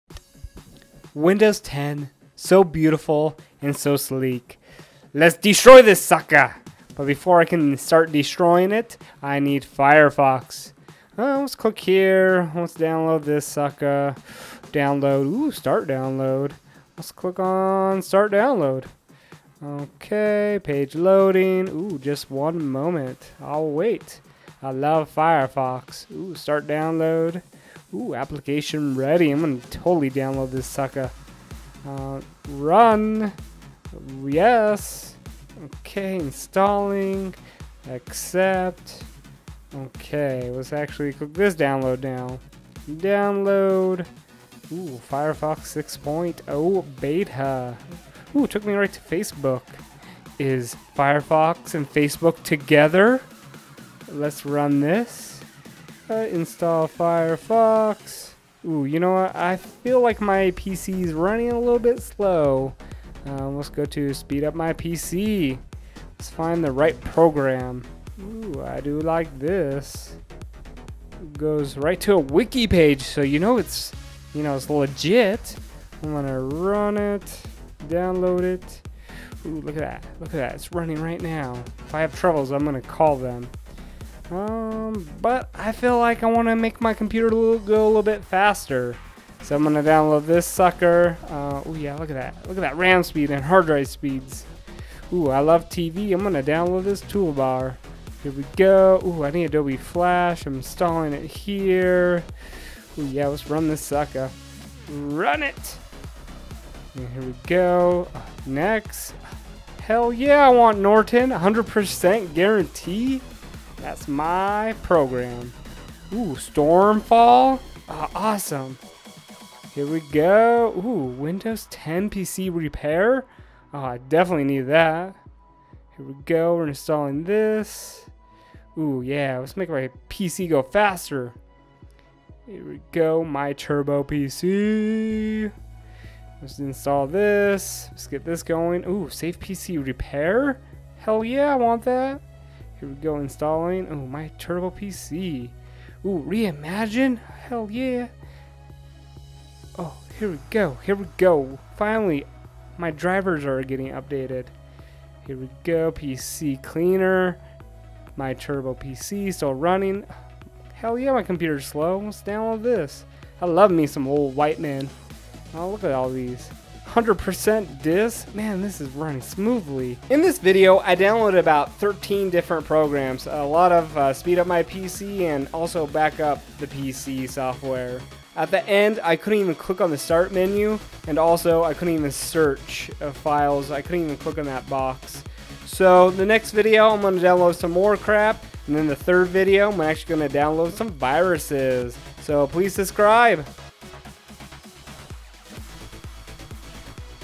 How many crappy programs can windows 10 take Part 1
I am trying to crash window 10 with random software. I been doing IT for 5 years now. The worst program people can download is anything that says it can speed up your computer. Don't get me wrong. There are software out there that can help your computer go faster (cclearner), but if they have speed up or boost in the name, it's going to cause more harm than good.
At the end of the video, The start menu wouldn't work.
Part 2 I will download more crap
and if window 10 is still working, Part 3 I will download virus.